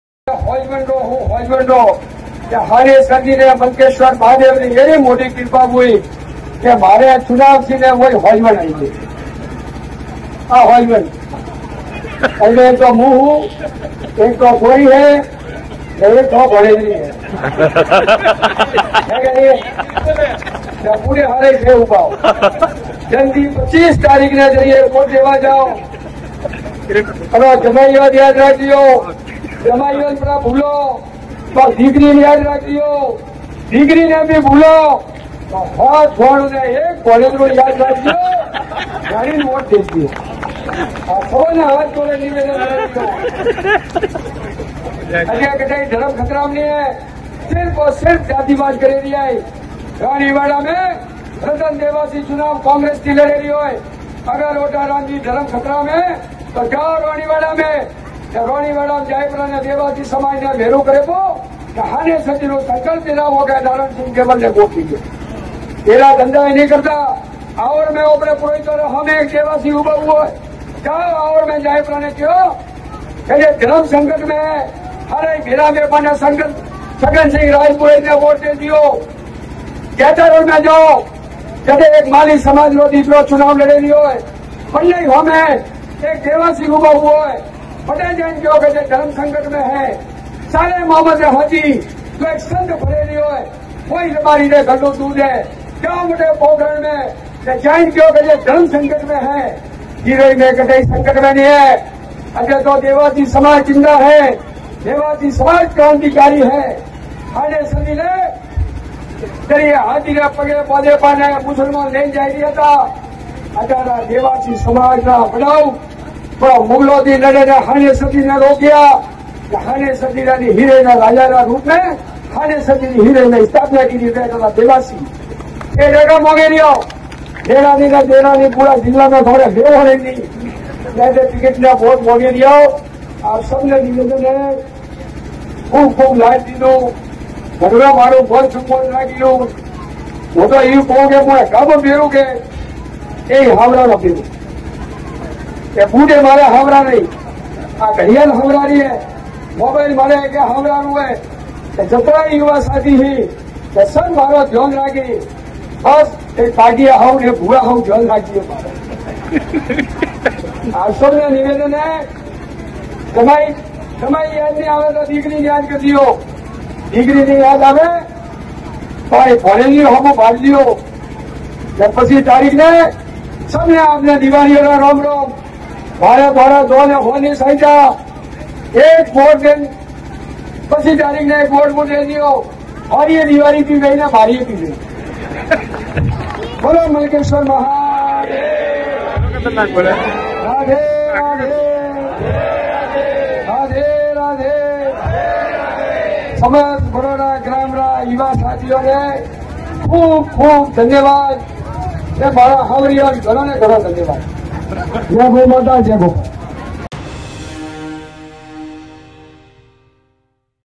Sirohi हेमंत पुरोहित vidhan sabha Viral Video Hemant Purohit ससुराल में अनोखे अंदाज में मांगे वोट
Sirohi vidhan sabha| Viral Video। Hemant Purohit  ने ससुराल में अनोखे अंदाज में मांगे वोट